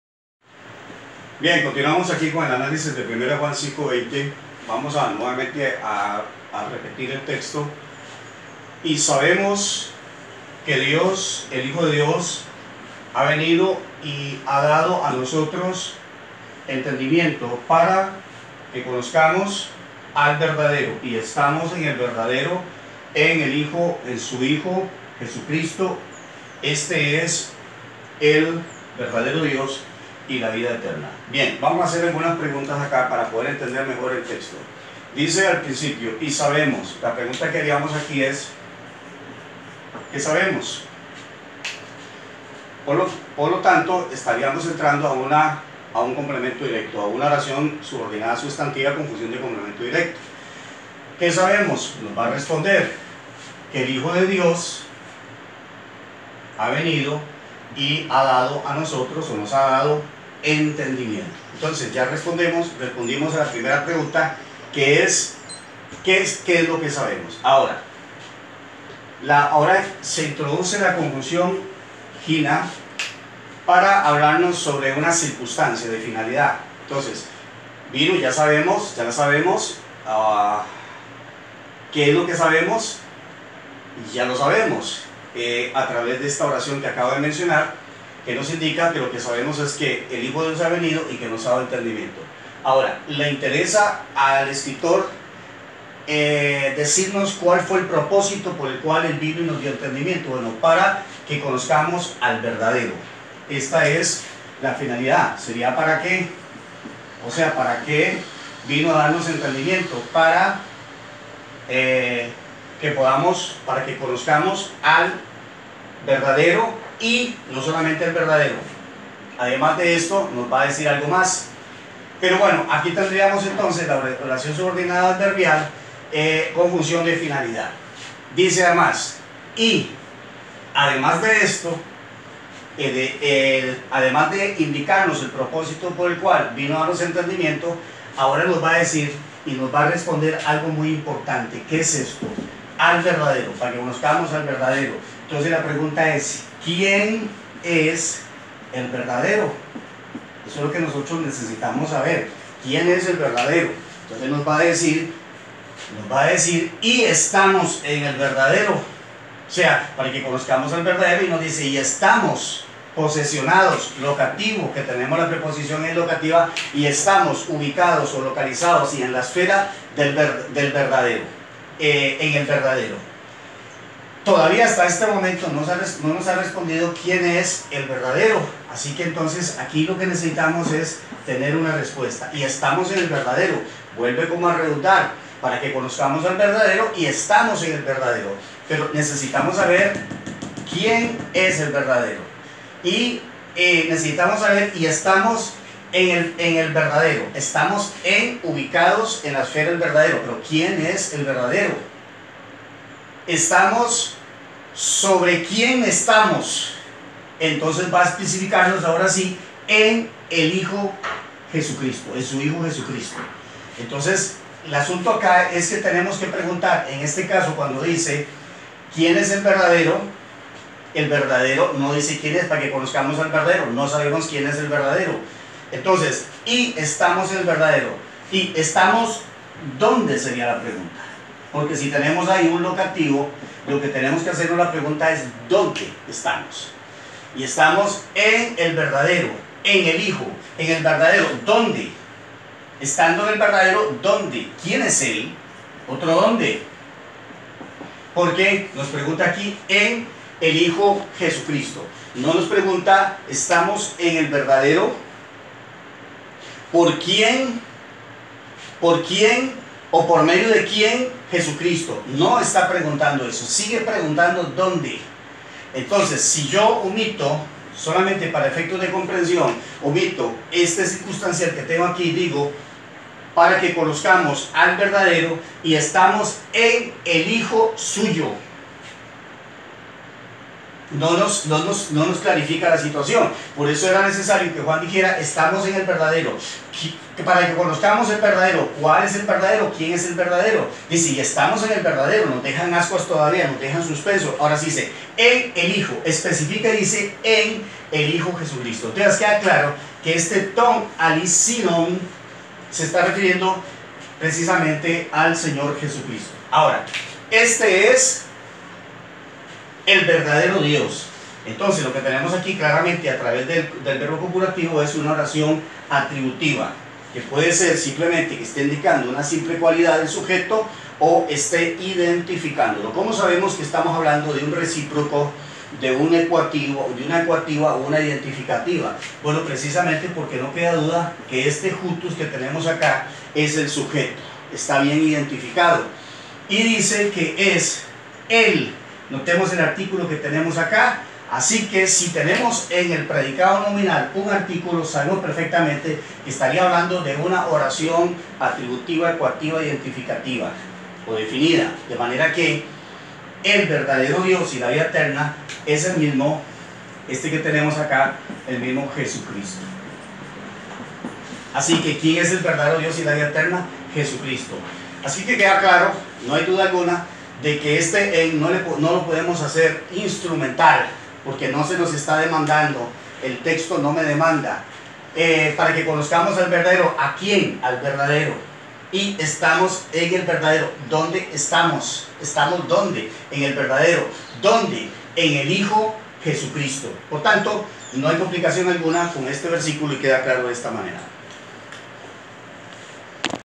Análisis bíblico del texto griego análisis sintáctico de IJn. 5:20.
Este texto nunca da como antecedente al Padre. JESÚS  es la
VIDA ETERNA!!!